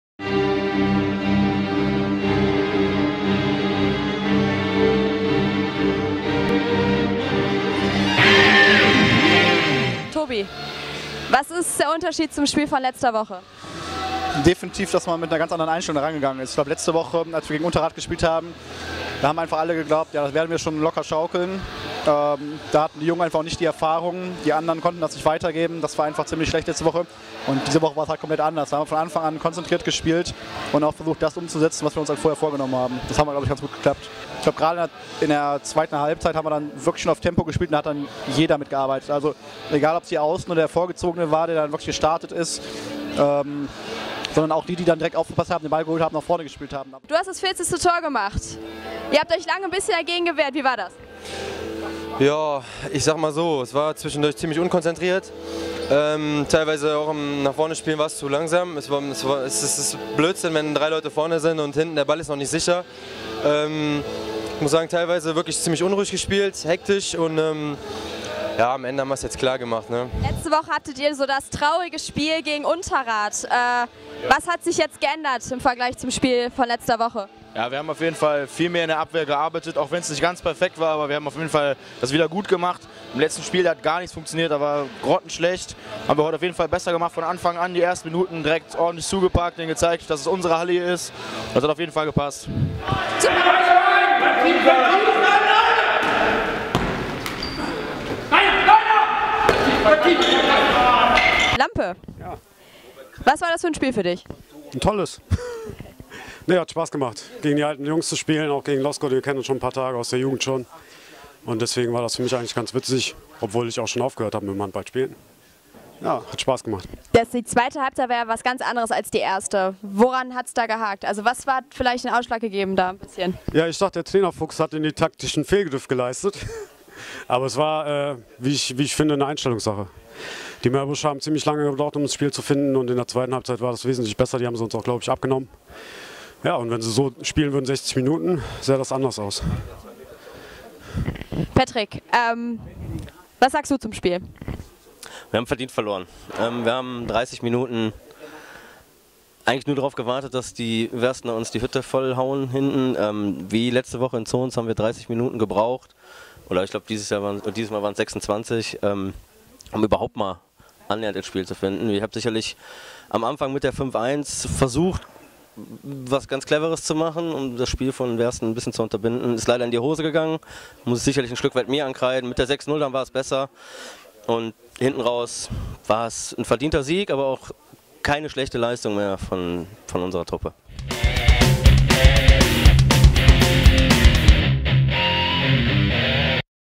7. Spieltag des MHV
Heimspieltag des MHV

MHV 1 - SV Wersten 04
MHV 2 - SG Ratingen 5